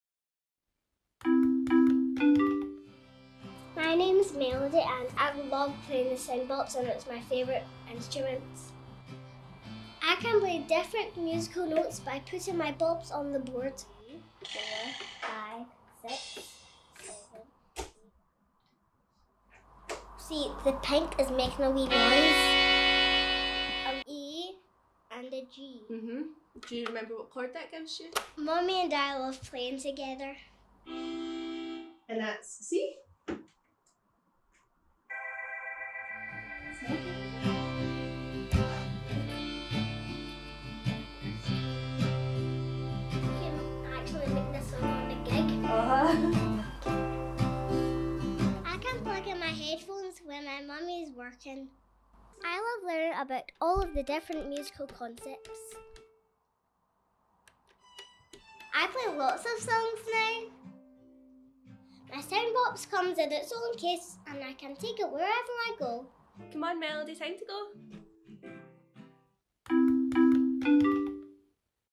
How Soundbops Works
Melodie is our unofficial tester and a big Soundbops fan. Here she is, describing how Soundbops works for her. And why it's her favourite instrument!